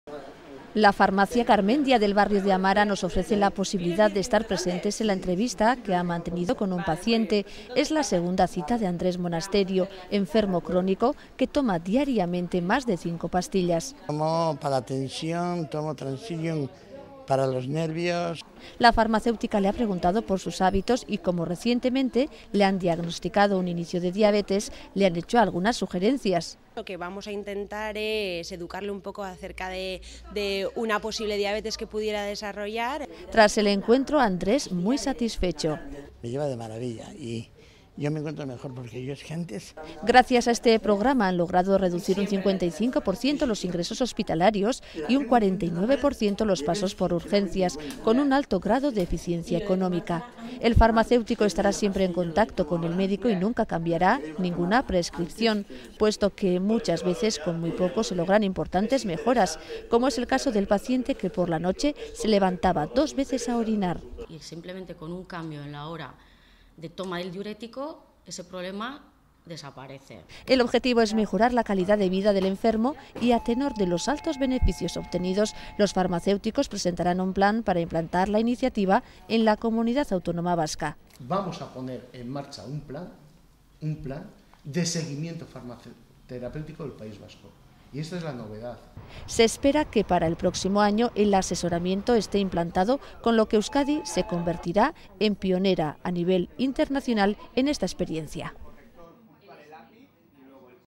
13 farmacias Gipuzkoanas harán un seguimiento a pacientes crónicos
13 farmacias Gipuzkoanas harán un seguimiento a pacientes crónicos.

Se trata del programa conSIGUE en la que participan más de 90 pacientes polimedicados. Un programa pionero que se quiere implantar en toda Euskadi para comienzos de año